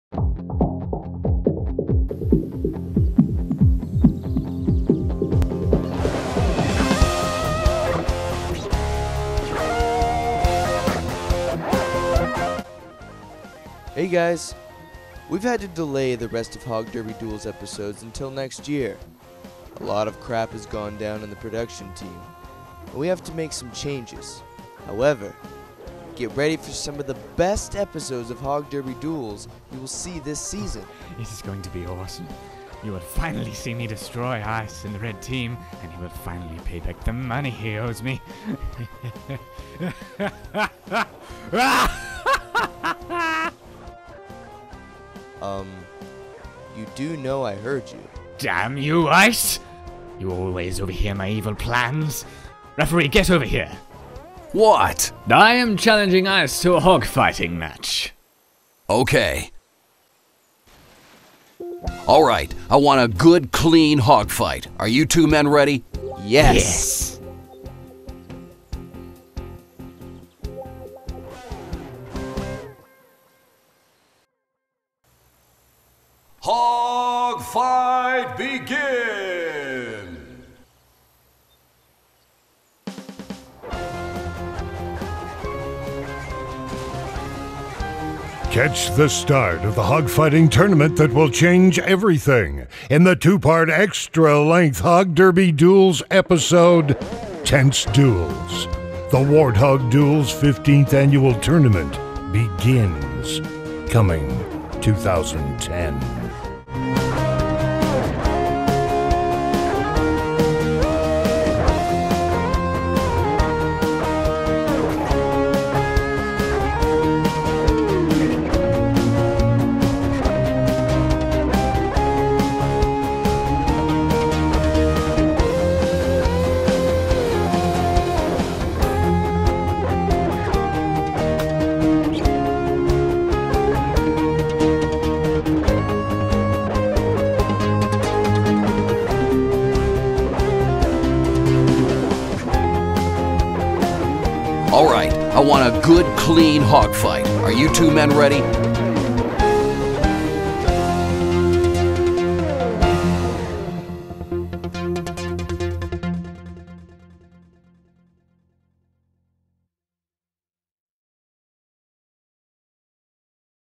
Hog Derby: Duels 2010 Update Video
Hog Derby: Duels is coming back later this year with new episodes going to Machinima.com with better voice acting, better writing and longer episodes!

The episodes for this year kick off with episode 4 and 5 which begin the "Warthog Duels 15th Annual Tournament" story arc for this season and will continue until the season finale.

Also...Want to help out the Hog Derby Duels team? We need cameraman and hog battle choreographers to start pumping this episodes out!

Trailers, behind-the-scenes and gagreels are all headed your way!

Dariusofwest
Hog Derby Productions